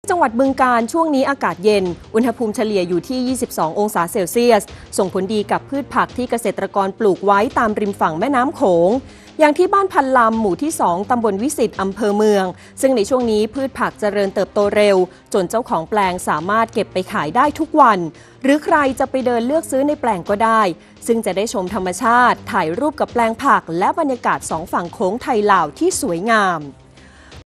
จ.บึงกาฬ อากาศเย็นพืชผักเติบโตดี ข่าวต้นชั่วโมง NBT เวลา 11.00 น. วันที่ 26 พฤศจิกายน 2567
ที่จังหวัดบึงกาฬ ช่วงนี้อากาศเย็น อุณหภูมิเฉลี่ยอยู่ที่ 22 องศาเซลเซียส ส่งผลดีกับพืชผักที่เกษตรกรปลูกไว้ตามริมฝั่งแม่น้ำโขง อย่างที่บ้านพันลำ หมู่ที่ 2 ตำบลวิศิษฐ์ อำเภอเมือง ซึ่งในช่วงนี้พืชผักเจริญเติบโตเร็ว จนเจ้าของแปลงสามารถเก็บไปขายได้ทุกวัน หรือใครจะไปเดินเลือกซื้อในแปลงก็ได้ ซึ่งจะได้ชมธรรมชาติ ถ่ายรูปกับแปลงผัก และบรรยากาศ 2 ฝั่งโขงไทย-ลาวที่สวยงาม